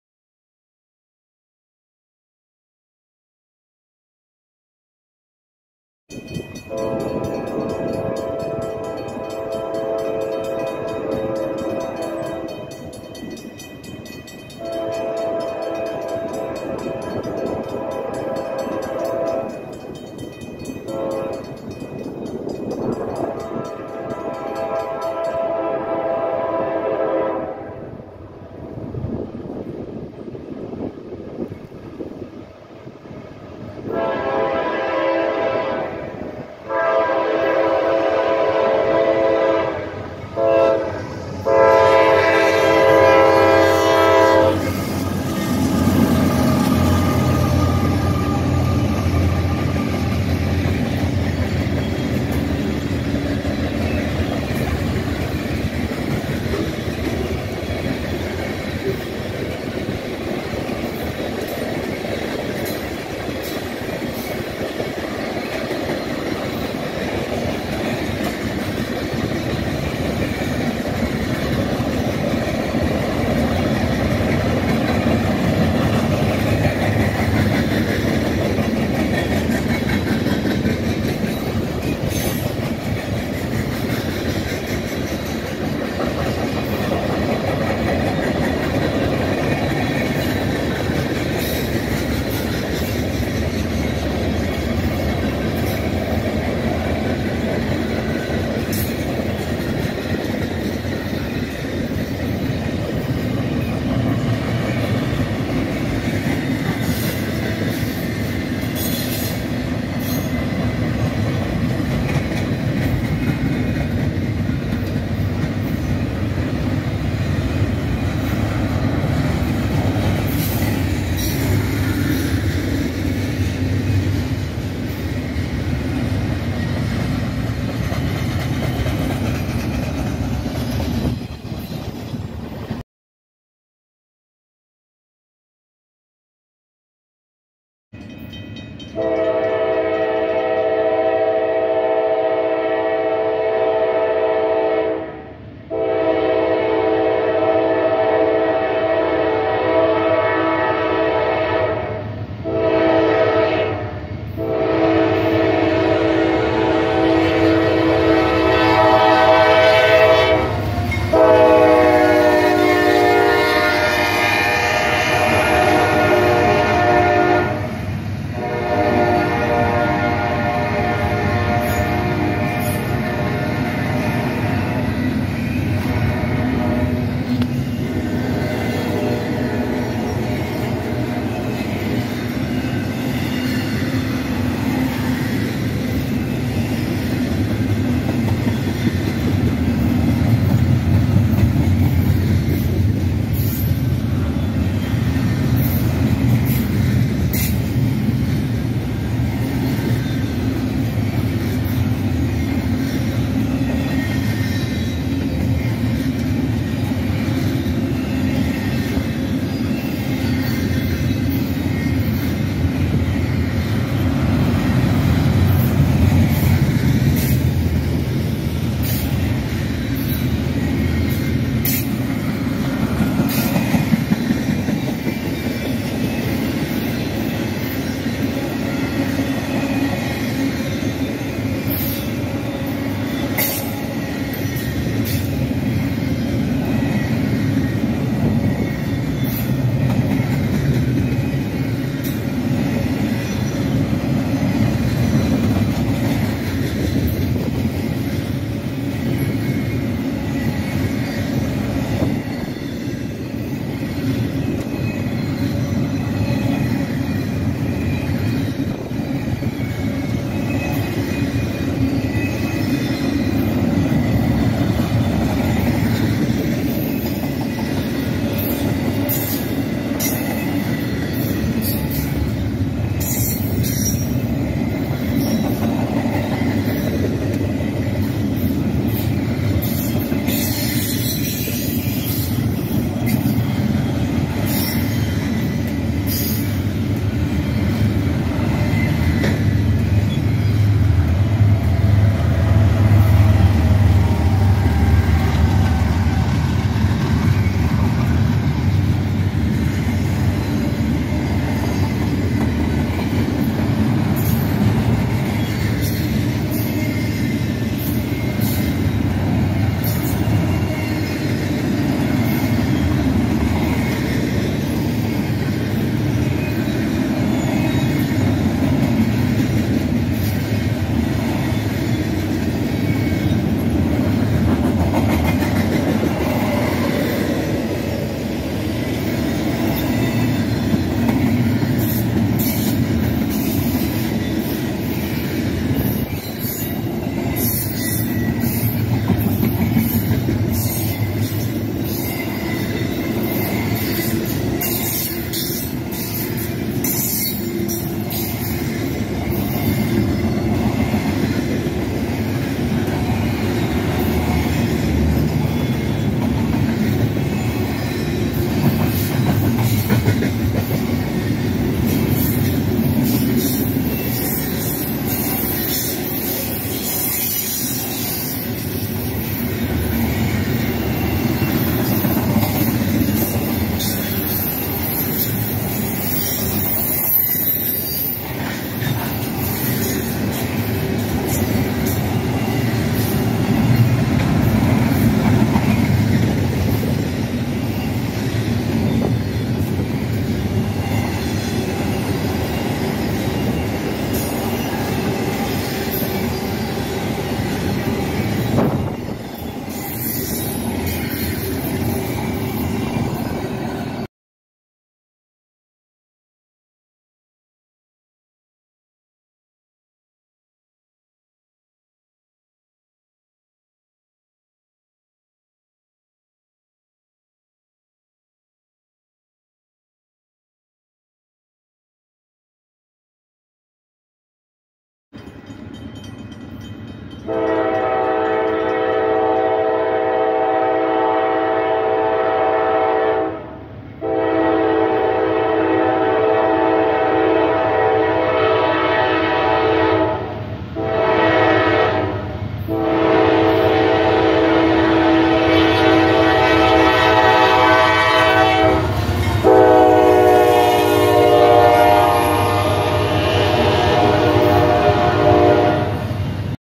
Heritage Classic "New York Central" #1066 trailing NS #4127 - 27a / NS #4313 - #242
Surprise Catch of the DAY!  #1066 NEW YORK CENTRAL Heritage Unit Hope YOU ENJOY!